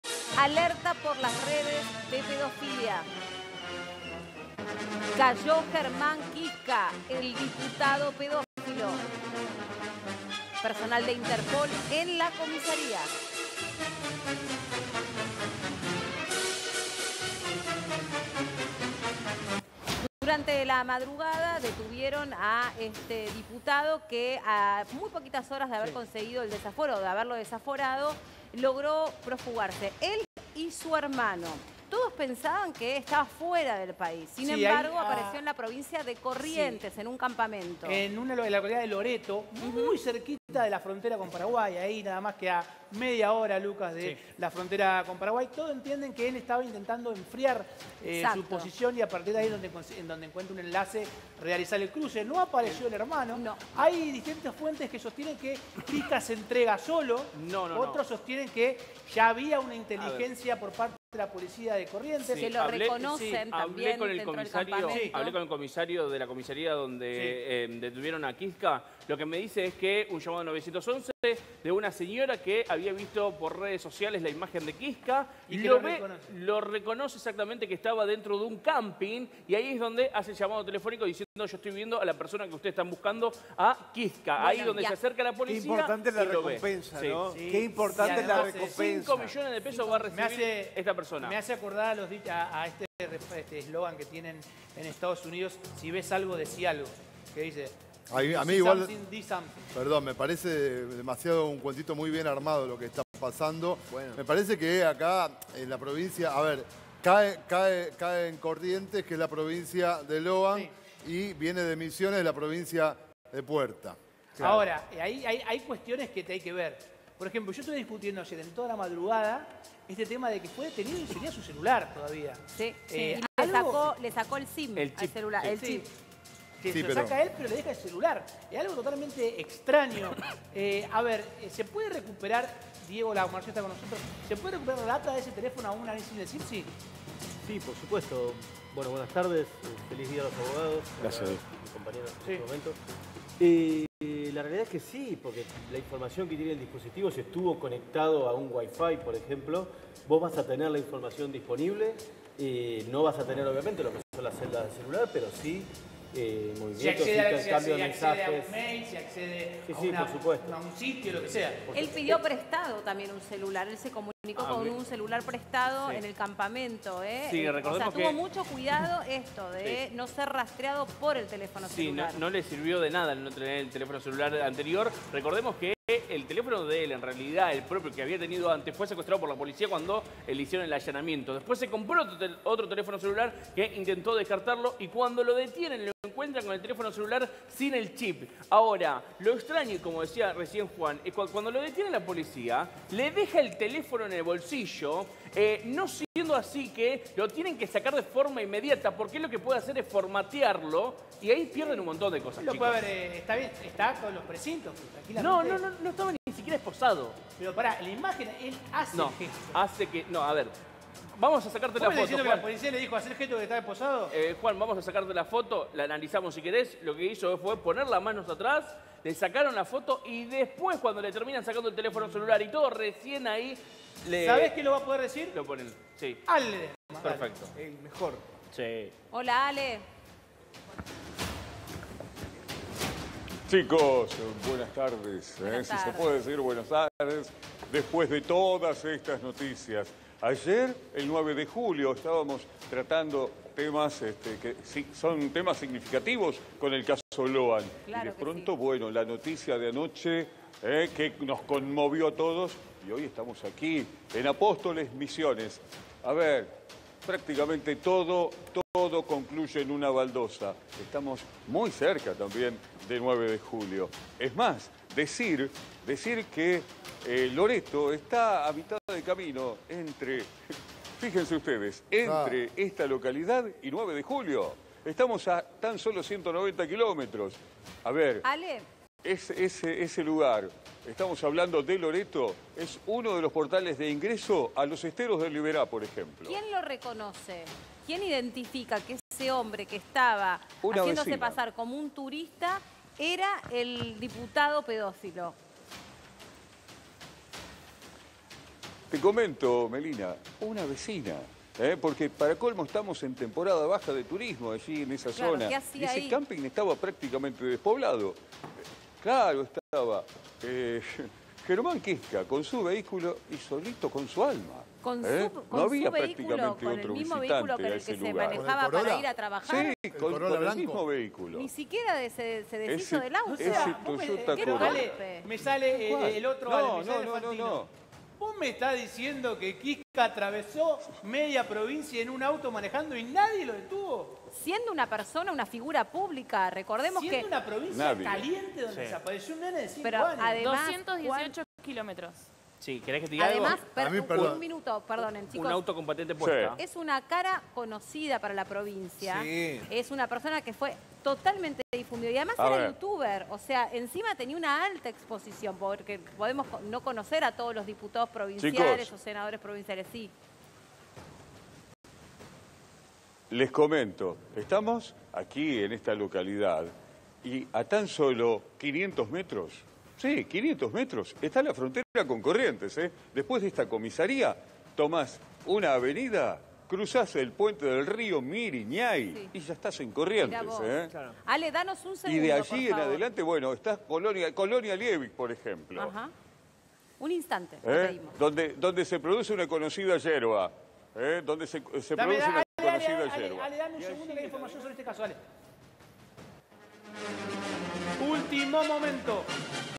Alerta por las redes de pedofilia: cayó Germán Kiczka, el diputado pedofilo
A muy pocas horas de conseguir el desafuero, el diputado y su hermano consiguieron profugarse, pero sin embargo apareció en un campamento en Corrientes, muy cerca de la frontera con Paraguay.